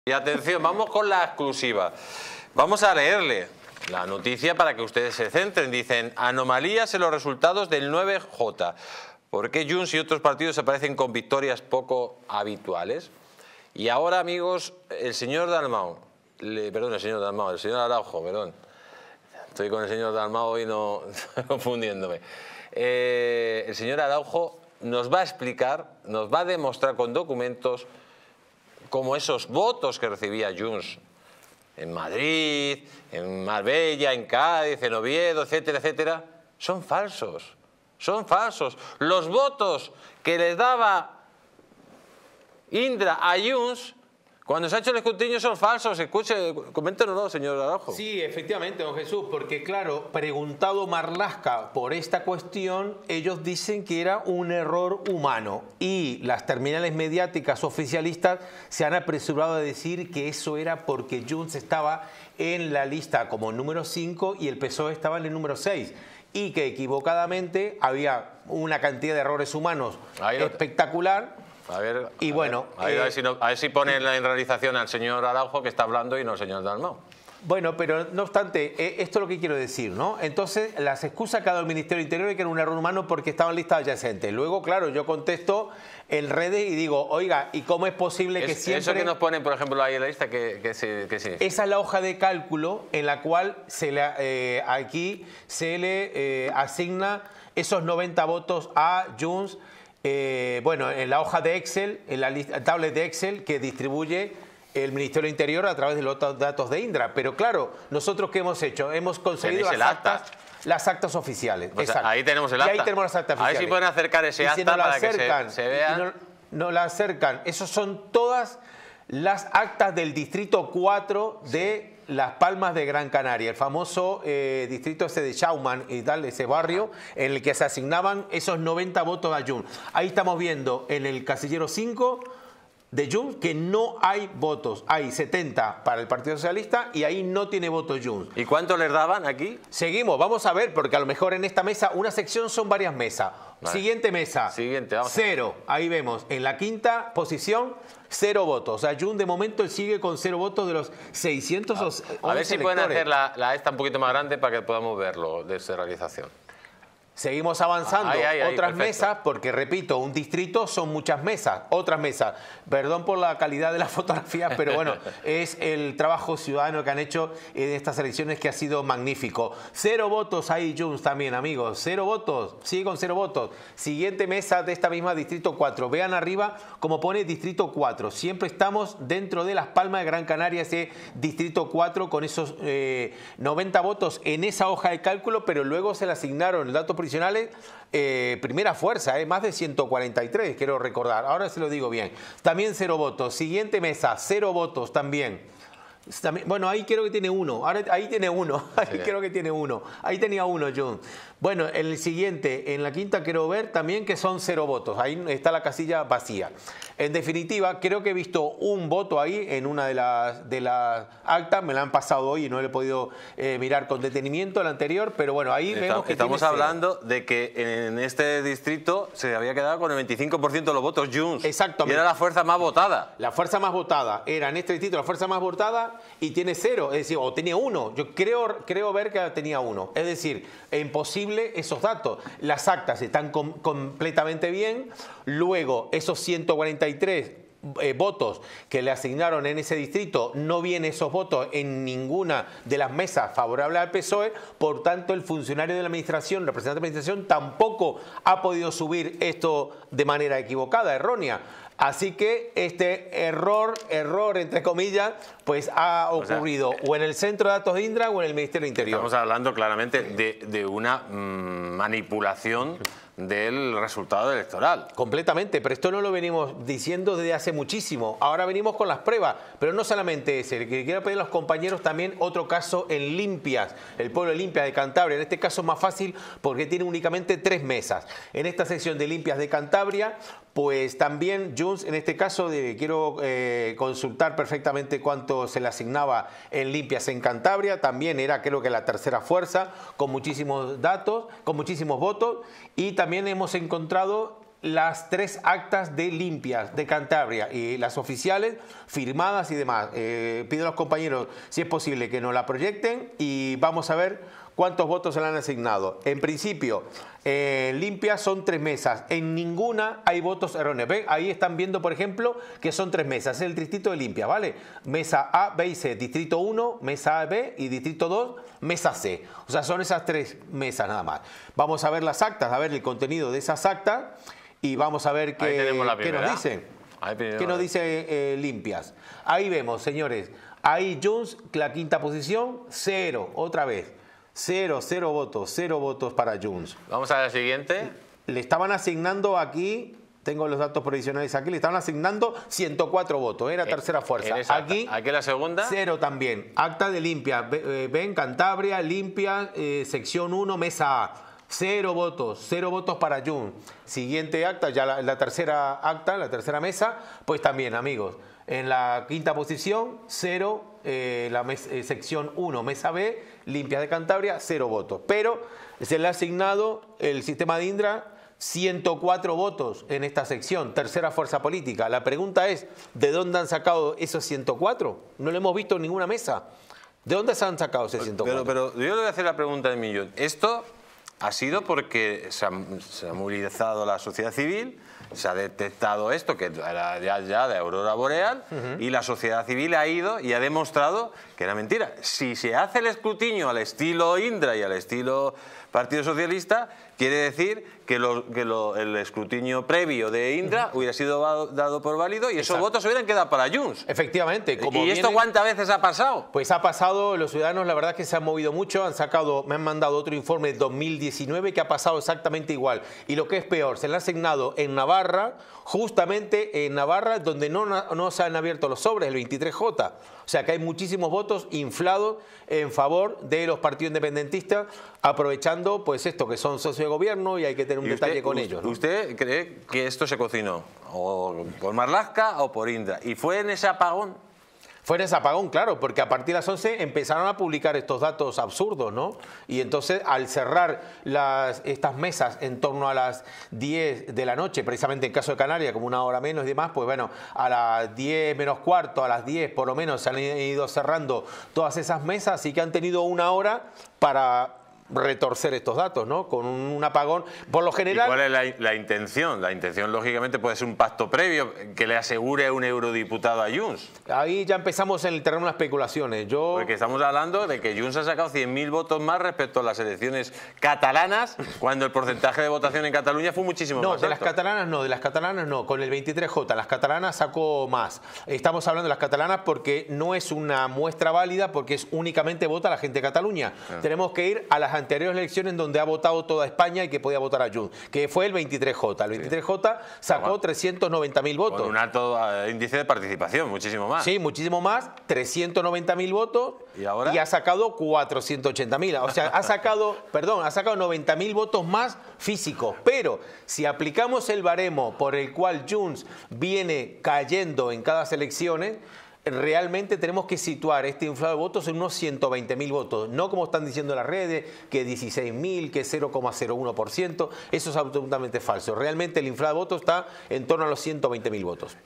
🔴EXCLUSIVA🔴 EL PERITO INFORMÁTICO SACA A LA LUZ LAS PRUEBAS DEL PUCHERAZO DE LOS VOTOS DE JUNTS🔴
En esta exclusiva, el perito informático revela pruebas del supuesto pucherazo en los votos de Junts. No te pierdas esta impactante revelación de DistritoTV.